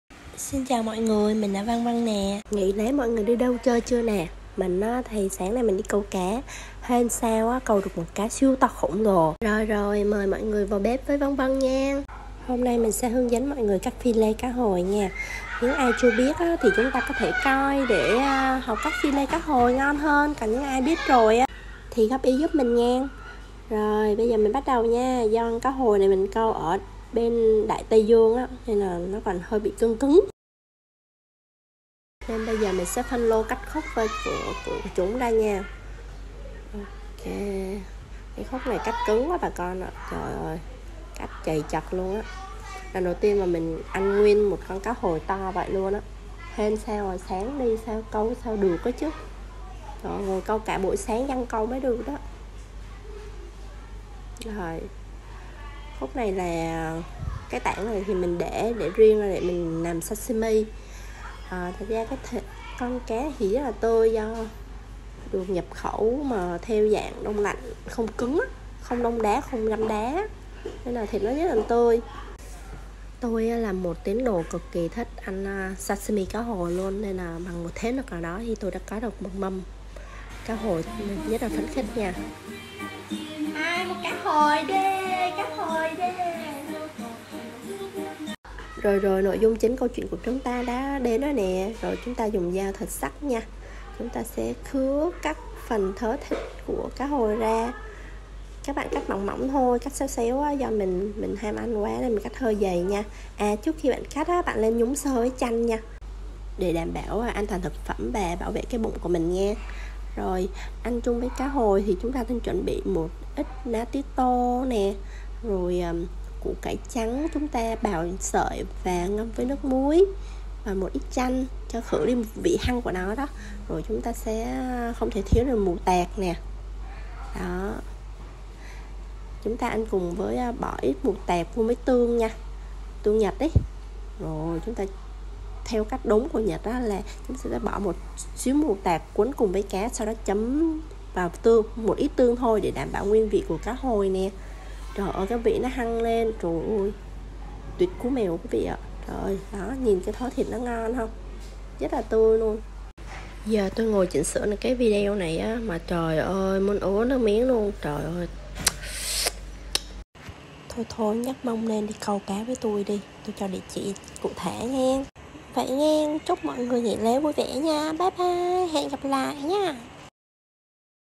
Cách làm sashimi cá hồi tươi nguyên con chuẩn vị Nhật
Cá hồi siêu to, siêu tươi